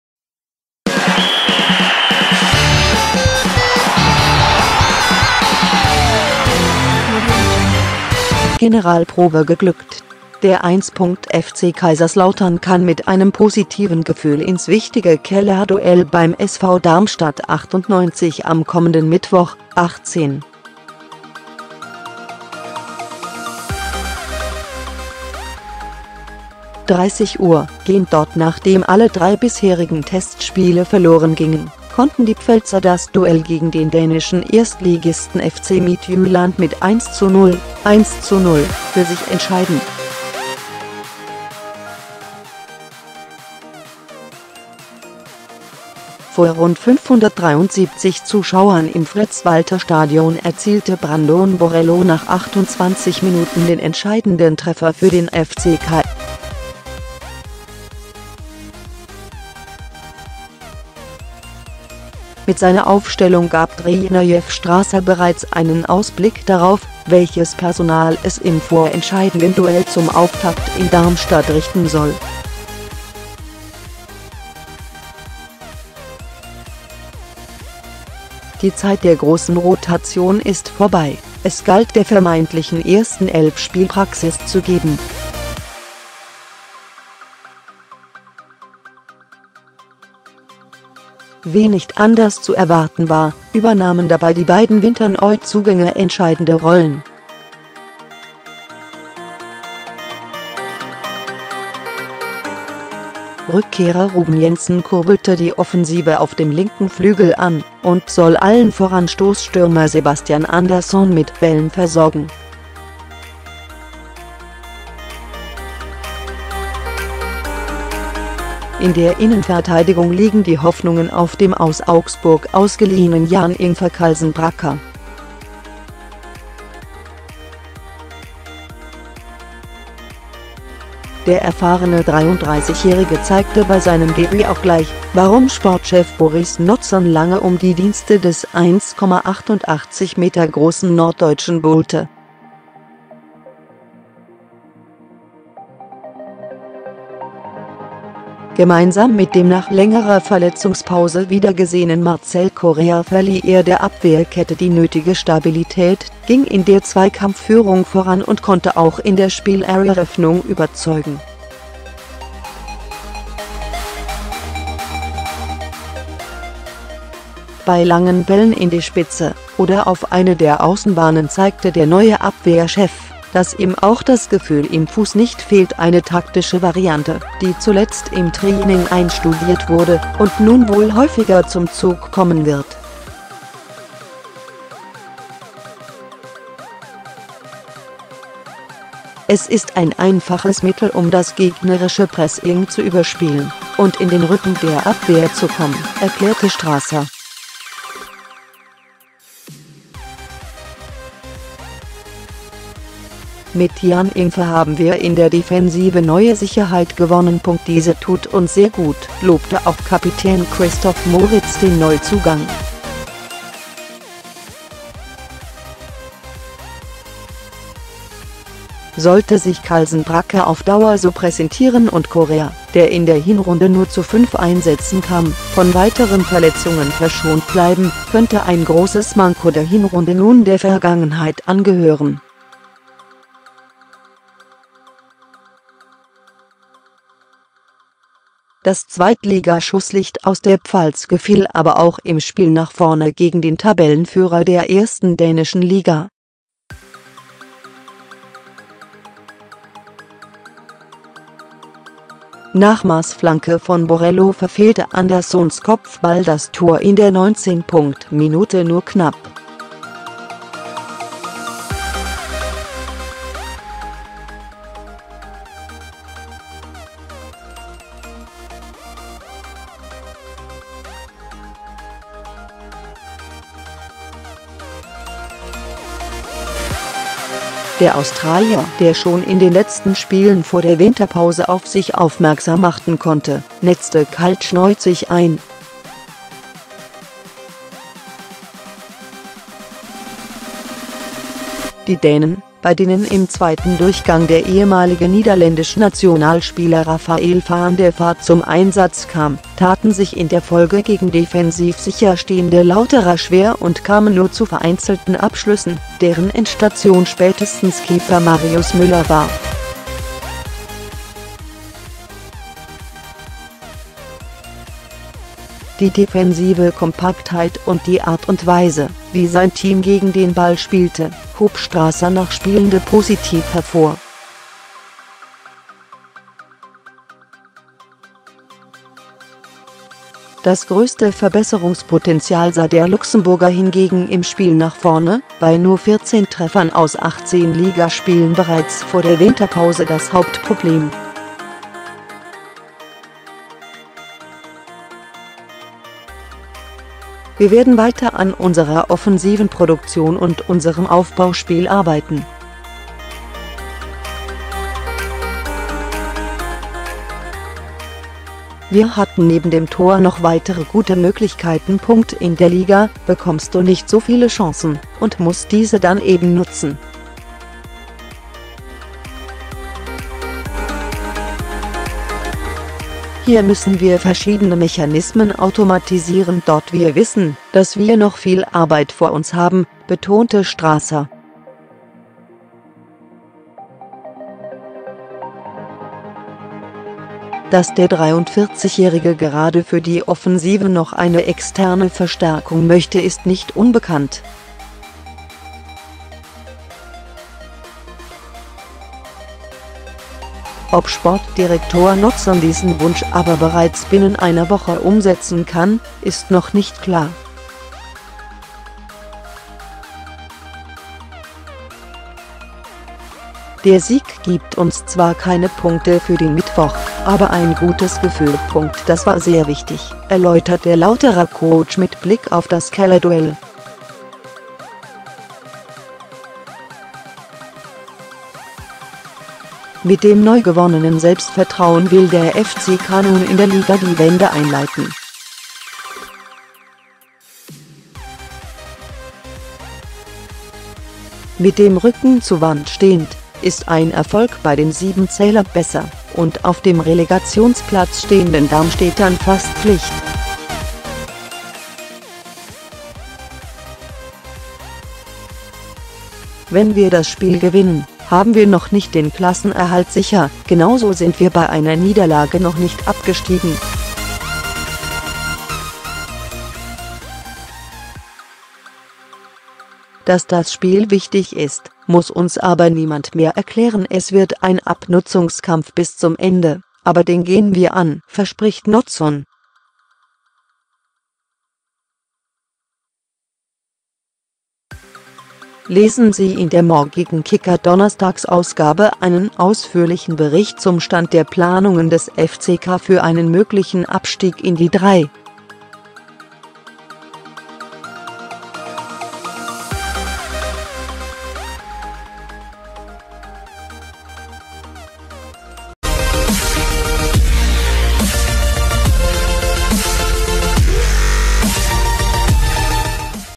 FC Midtjylland 1:0, Fußball-Vereine Freundschaftsspiele, Saison 2017/18, 29.Spieltag
Generalprobe geglückt: Der 1.FC Kaiserslautern kann mit einem positiven Gefühl ins wichtige Kellerduell beim SV Darmstadt 98 am kommenden Mittwoch (18.30 Uhr) gehen.Nachdem alle drei bisherigen Testspiele verloren gingen, konnten die Pfälzer das Duell gegen den dänischen Erstligisten FC Midtjylland mit 1:0 (1:0) für sich entscheiden.Vor rund 573 Zuschauern im Fritz-Walter-Stadion erzielte Brandon Borrello nach 28 Minuten den entscheidenden Treffer für den FCK.Mit seiner Aufstellung gab Trainer Jeff Strasser bereits einen Ausblick darauf, welches Personal es im vorentscheidenden Duell zum Auftakt in Darmstadt richten soll.Die Zeit der großen Rotation ist vorbei, es galt der vermeintlichen ersten Elf Spielpraxis zu geben.Wie nicht anders zu erwarten war, übernahmen dabei die beiden Winterneuzugänge entscheidende Rollen.Rückkehrer Ruben Jenssen kurbelte die Offensive auf dem linken Flügel an und soll allen voran Stoßstürmer Sebastian Andersson mit Bällen versorgen.In der Innenverteidigung liegen die Hoffnungen auf dem aus Augsburg ausgeliehenen Jan-Ingwer Callsen-Bracker.Der erfahrene 33-Jährige zeigte bei seinem Debüt auch gleich, warum Sportchef Boris Notzon lange um die Dienste des 1,88 Meter großen Norddeutschen buhlte.Gemeinsam mit dem nach längerer Verletzungspause wiedergesehenen Marcel Correia verlieh er der Abwehrkette die nötige Stabilität, ging in der Zweikampfführung voran und konnte auch in der Spielereröffnung überzeugen.Bei langen Bällen in die Spitze oder auf eine der Außenbahnen zeigte der neue Abwehrchef, dass ihm auch das Gefühl im Fuß nicht fehlt - eine taktische Variante, die zuletzt im Training einstudiert wurde und nun wohl häufiger zum Zug kommen wird."Es ist ein einfaches Mittel um das gegnerische Pressing zu überspielen und in den Rücken der Abwehr zu kommen", erklärte Strasser."Mit Jan-Ingwer haben wir in der Defensive neue Sicherheit gewonnen.Diese tut uns sehr gut", lobte auch Kapitän Christoph Moritz den Neuzugang.Sollte sich Callsen-Bracker auf Dauer so präsentieren und Correia, der in der Hinrunde nur zu fünf Einsätzen kam, von weiteren Verletzungen verschont bleiben, könnte ein großes Manko der Hinrunde nun der Vergangenheit angehören.Das Zweitliga-Schusslicht aus der Pfalz gefiel aber auch im Spiel nach vorne gegen den Tabellenführer der ersten dänischen Liga.Nach Maßflanke von Borrello verfehlte Anderssons Kopfball das Tor in der 19.Minute nur knapp.In getauschten Rollen klappte es neun Minuten später dann besser: Einen langen Ball Correias nahm Andersson mit bis zur Grundlinie und legte von dort zurück auf Borrello.Der Australier, der schon in den letzten Spielen vor der Winterpause auf sich aufmerksam machten konnte, netzte kaltschnäuzig ein.Die Dänen, bei denen im zweiten Durchgang der ehemalige Niederländische Nationalspieler Rafael van der Vaart zum Einsatz kam, taten sich in der Folge gegen defensiv sicher stehende Lauterer schwer und kamen nur zu vereinzelten Abschlüssen, deren Endstation spätestens Keeper Marius Müller war.Die defensive Kompaktheit und die Art und Weise, wie sein Team gegen den Ball spielte, hob Strasser nach Spielende positiv hervor.Das größte Verbesserungspotenzial sah der Luxemburger hingegen im Spiel nach vorne, bei nur 14 Treffern aus 18 Ligaspielen bereits vor der Winterpause das Hauptproblem."Wir werden weiter an unserer offensiven Produktion und unserem Aufbauspiel arbeiten.Wir hatten neben dem Tor noch weitere gute Möglichkeiten.In der Liga bekommst du nicht so viele Chancen und musst diese dann eben nutzen.Hier müssen wir verschiedene Mechanismen automatisieren.Wir wissen, dass wir noch viel Arbeit vor uns haben", betonte Strasser.Dass der 43-Jährige gerade für die Offensive noch eine externe Verstärkung möchte ist nicht unbekannt.Ob Sportdirektor Notzon diesen Wunsch aber bereits binnen einer Woche umsetzen kann, ist noch nicht klar."Der Sieg gibt uns zwar keine Punkte für den Mittwoch, aber ein gutes Gefühl.Das war sehr wichtig", erläutert der Lauterer Coach mit Blick auf das Kellerduell.Mit dem neu gewonnenen Selbstvertrauen will der FCK nun in der Liga die Wende einleiten.Mit dem Rücken zu Wand stehend, ist ein Erfolg bei den sieben Zähler besser und auf dem Relegationsplatz stehenden Darmstädtern fast Pflicht."Wenn wir das Spiel gewinnen, haben wir noch nicht den Klassenerhalt sicher, genauso sind wir bei einer Niederlage noch nicht abgestiegen.Dass das Spiel wichtig ist, muss uns aber niemand mehr erklären Es wird ein Abnutzungskampf bis zum Ende, aber den gehen wir an", verspricht Notzon.Lesen Sie in der morgigen kicker-Donnerstagsausgabe einen ausführlichen Bericht zum Stand der Planungen des FCK für einen möglichen Abstieg in die 3.